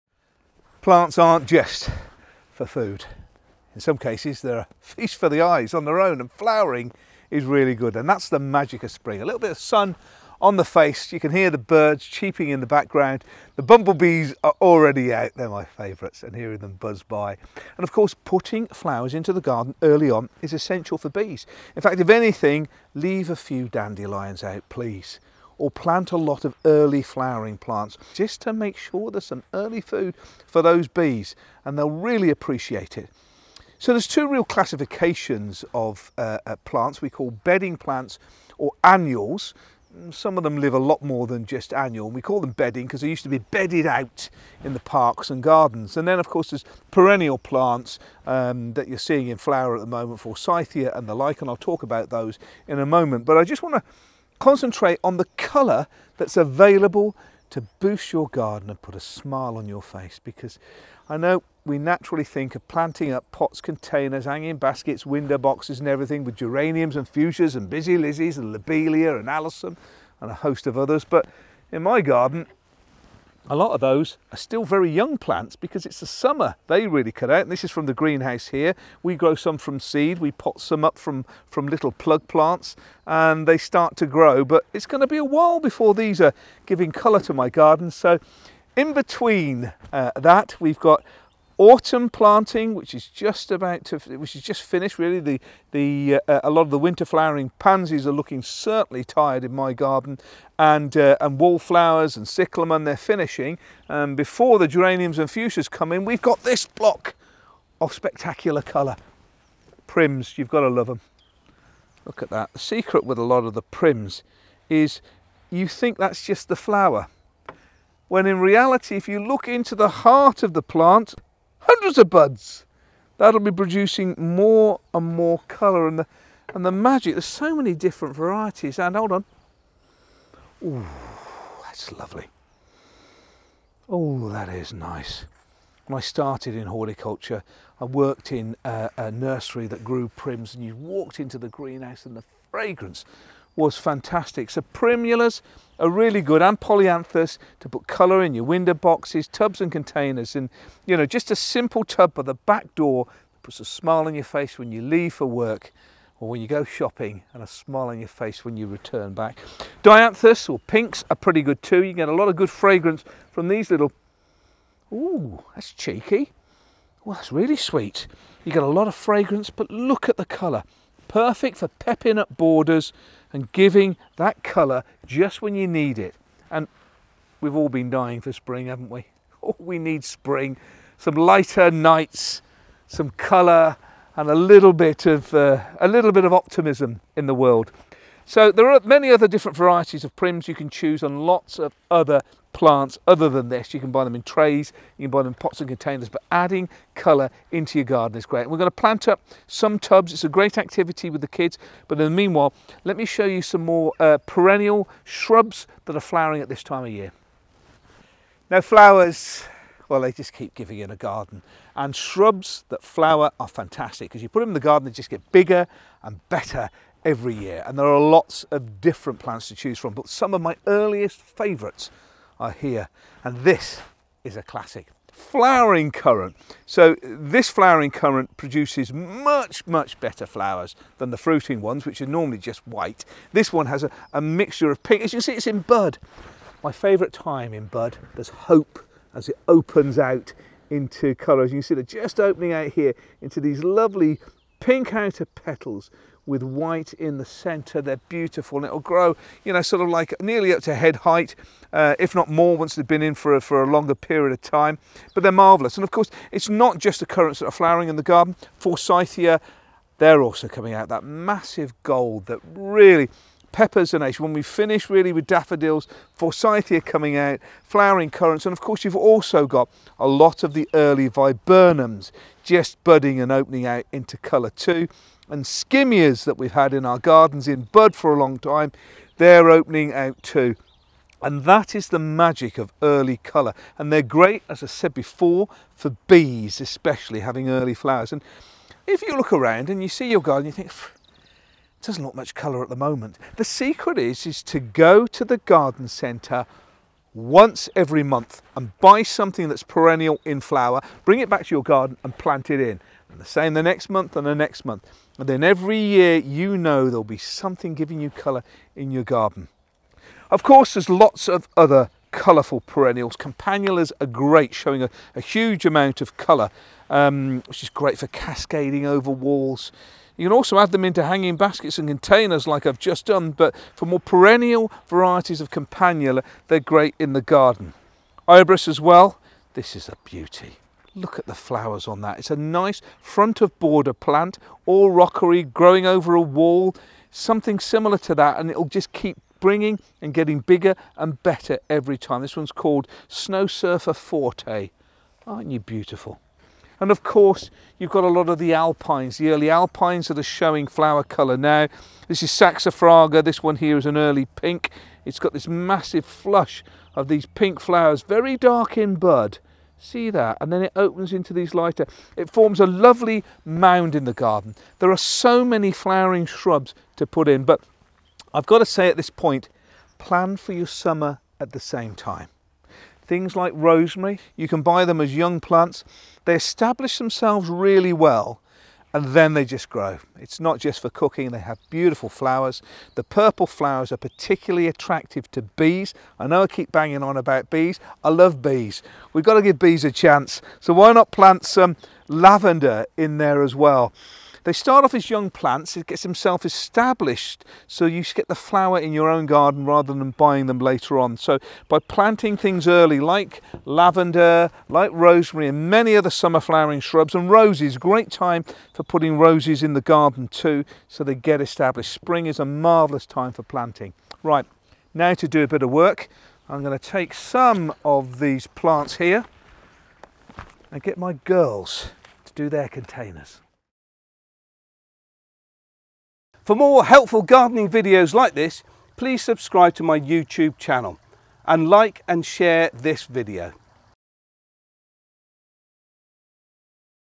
Perfect plants for spring colour in the garden
A question I am often asked is, what plants are perfect for a spring colour display in the garden? Well in the UK there is so much great choice. And the blooms that do appear can offer a lush spring display of colourful flowers that make any garden, large or small look spectacular.